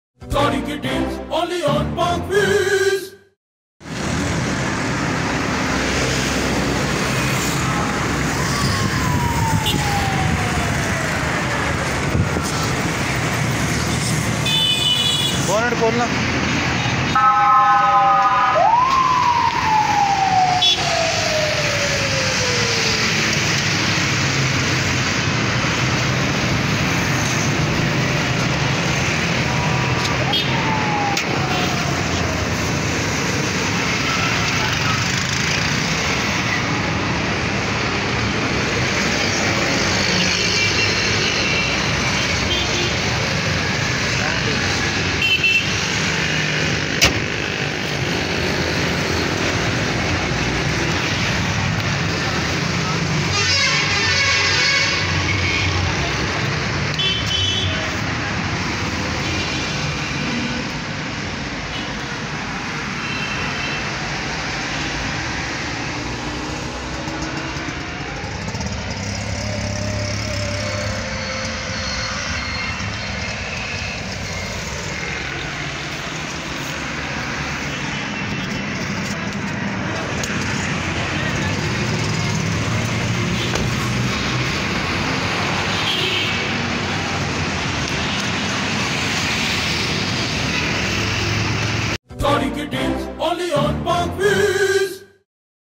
Toyota Prius A Touring Selection 2017 for Sale
2017/2020
Just like a Zero Meter car.
Price is flexible.
Totally driven on petrol.
No call/SMS will be answered after office hours.